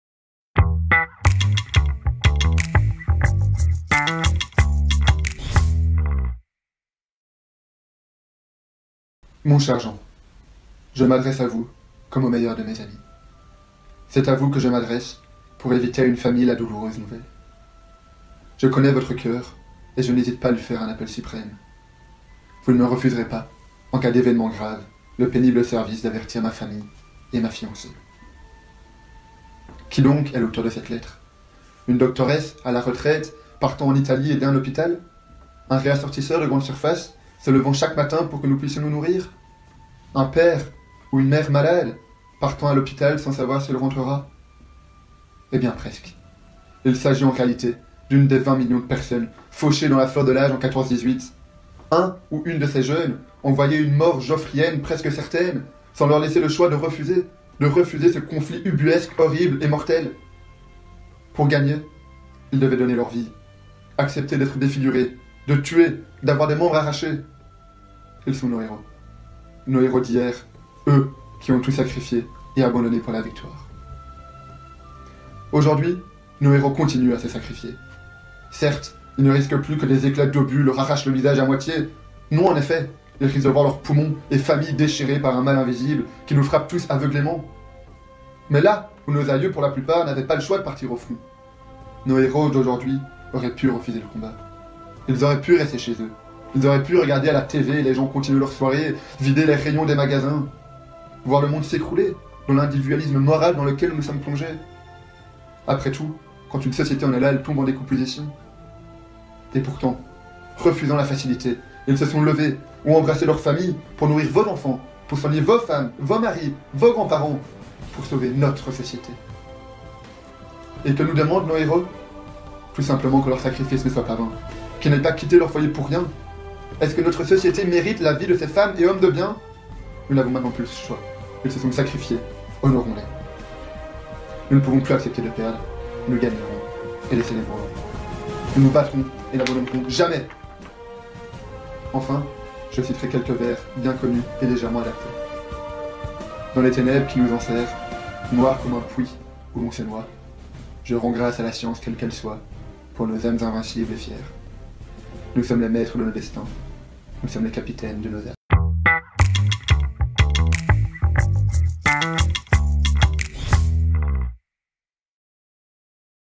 Lockdown Challenge | We shall fight and never surrender
Corentin Dardenne est étudiant à l’ULB mais est également passionné d’histoire et de philosophie 📖

À travers sa capsule vidéo, il rend hommage à l'histoire, aux héros invaincus et nous invite à ne pas cesser nos efforts 👊

Qui sera le prochain à s'exprimer ? 📣